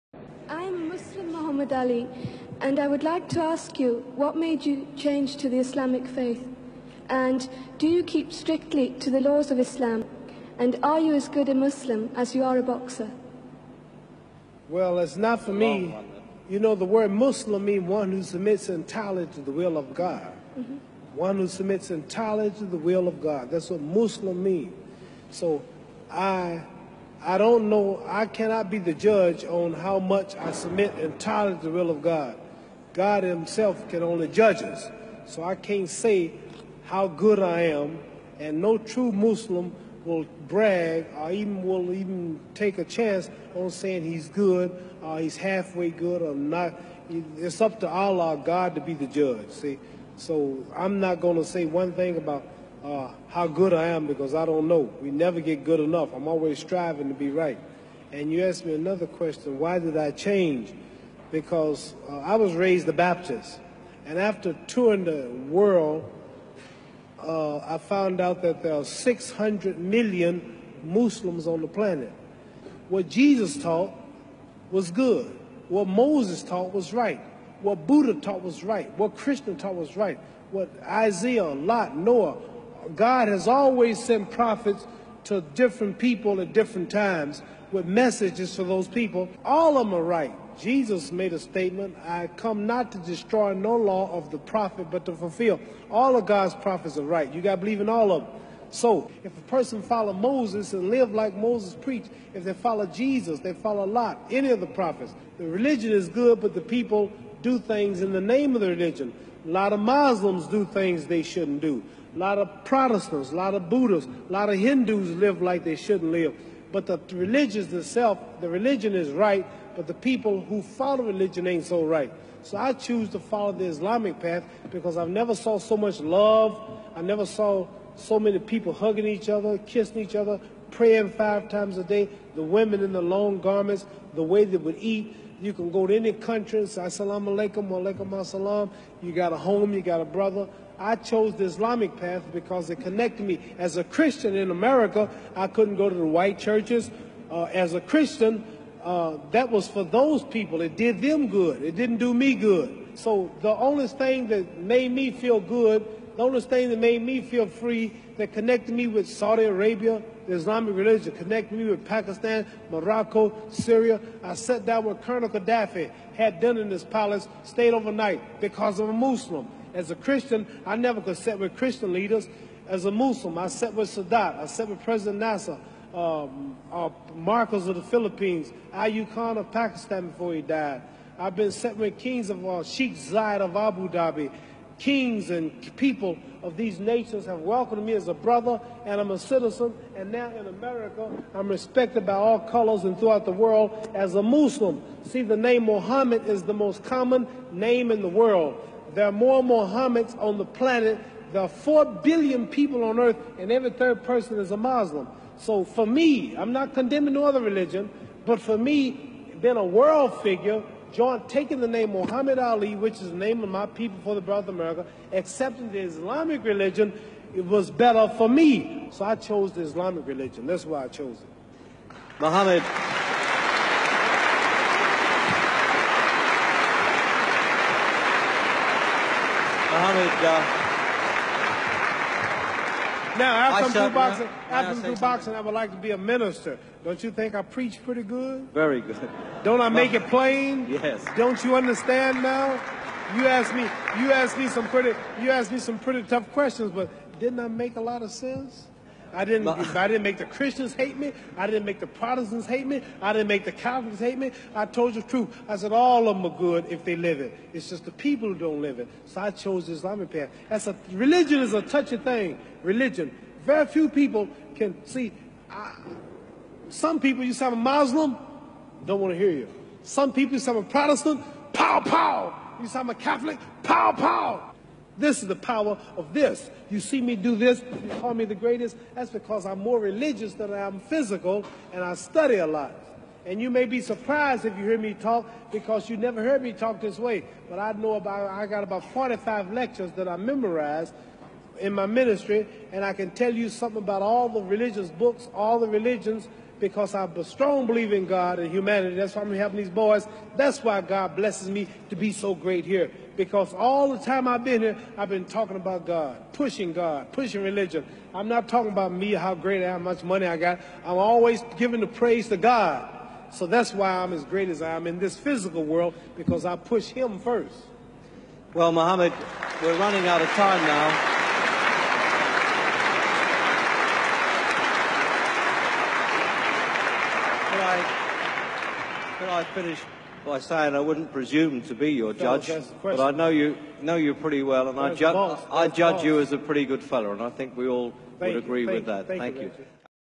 Muhammed Ali says | why he became a Muslim - amazing and inspirational tribute likes
Muhammed Ali explains why he is the greatest. Why he entered the fold of Islam and we humans have no right to judge anyone, Only Allah (God) is the Judg